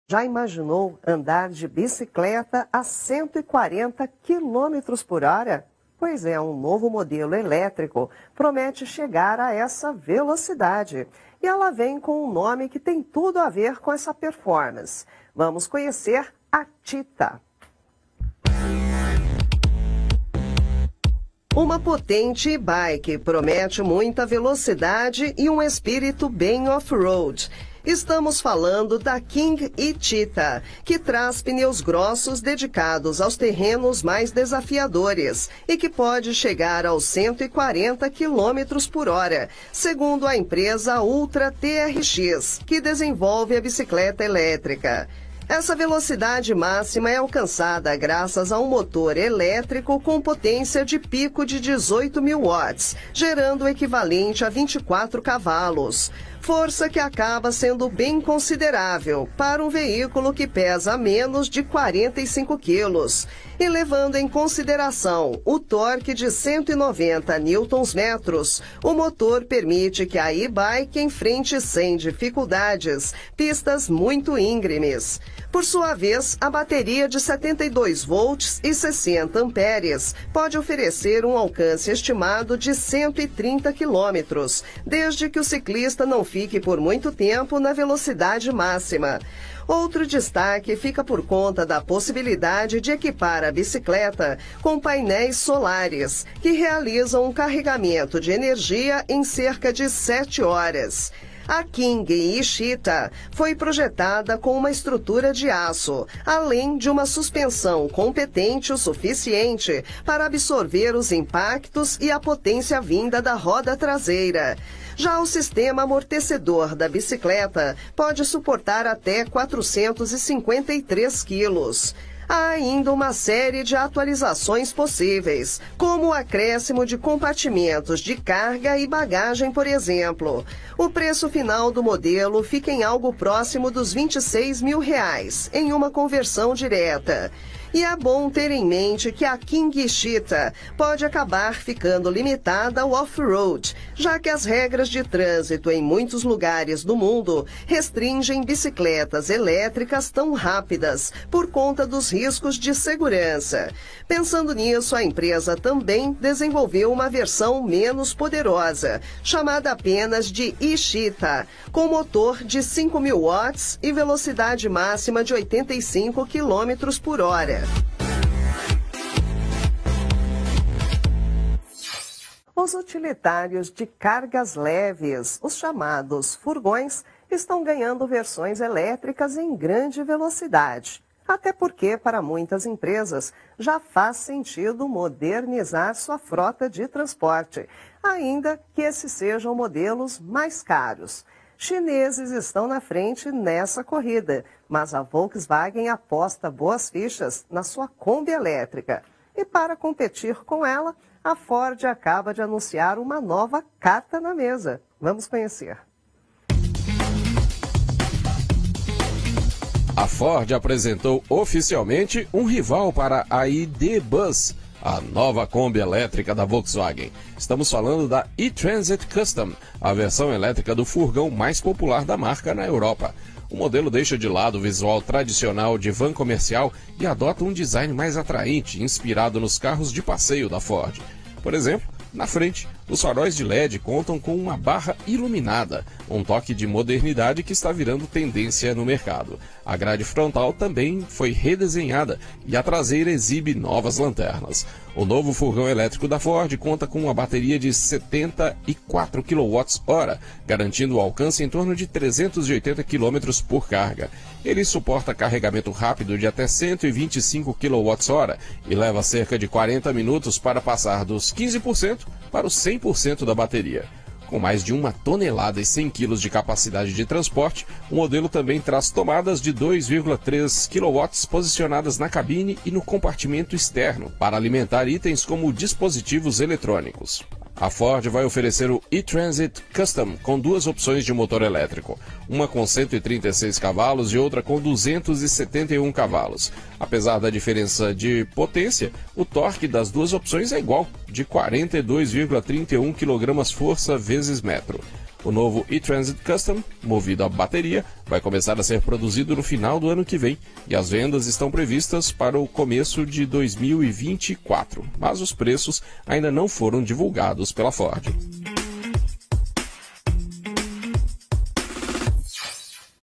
King E-Cheetah: bicicleta elétrica é capaz de atingir 140 km/h
O segmento de bicicletas elétricas continua aquecido no exterior, com modelos menos caros e outros cada vez mais potentes. Essa é a proposta da King E-Cheetah, uma e-bike "turbinada" capaz de atingir 140 km/h.